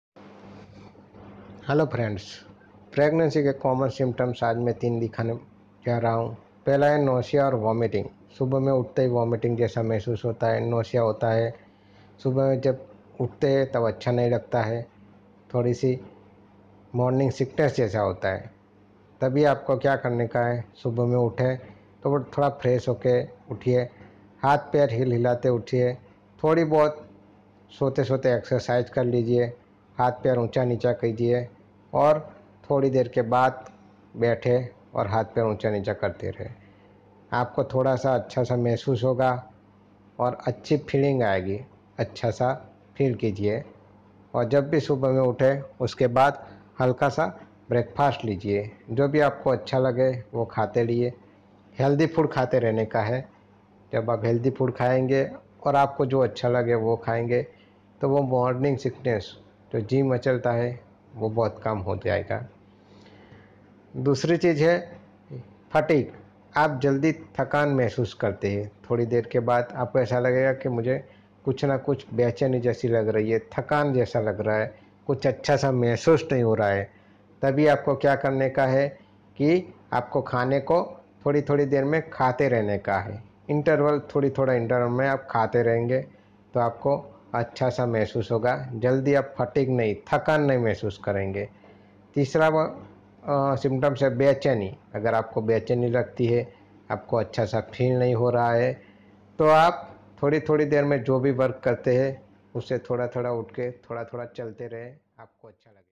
Pregnancy symptoms and remedies
How to decrease comman symptoms of pregnancy, without medicine